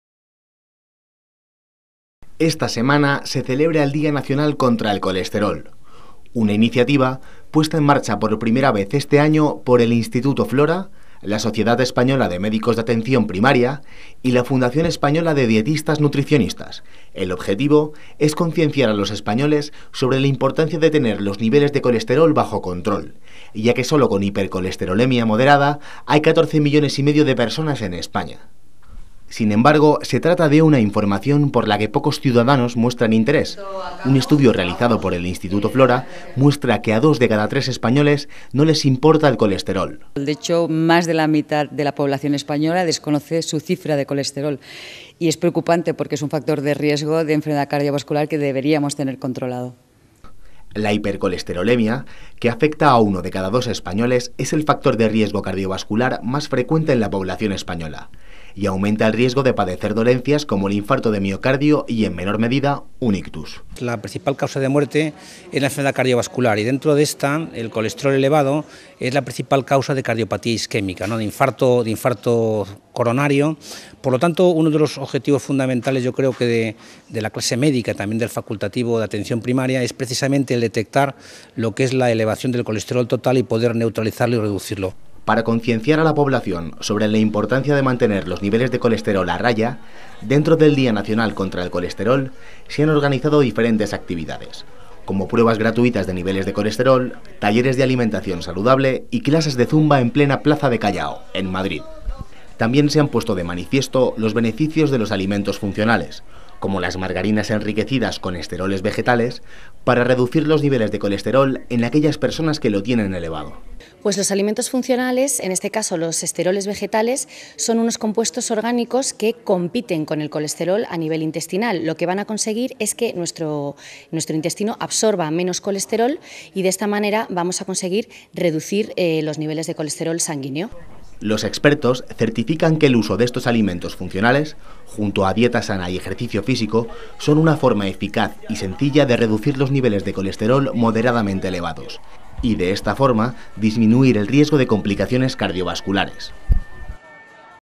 Primer Día Nacional contra el Colesterol
Hoy se celebra el Día Nacional contra el Colesterol, una iniciativa puesta en marcha por primera vez este año por el Instituto Flora, la Sociedad Española de Médicos de Atención Primaria y la Fundación Española de Dietistas-Nutricionistas. El objetivo es concienciar a los españoles sobre la importancia de tener los niveles de colesterol bajo control, ya que sólo con hipercolesterolemia moderada hay catorce millones y medio de personas en España.

Sin embargo, se trata de una información por la que pocos ciudadanos muestran interés. Un estudio realizado por el Instituto Flora muestra que a dos de cada tres españoles no les importa el colesterol. La hipercolesterolemia, que afecta a uno de cada dos españoles, es el factor de riesgo cardiovascular más frecuente en la población española y aumenta el riesgo de padecer dolencias como el infarto de miocardio y, en menor medida, un ictus.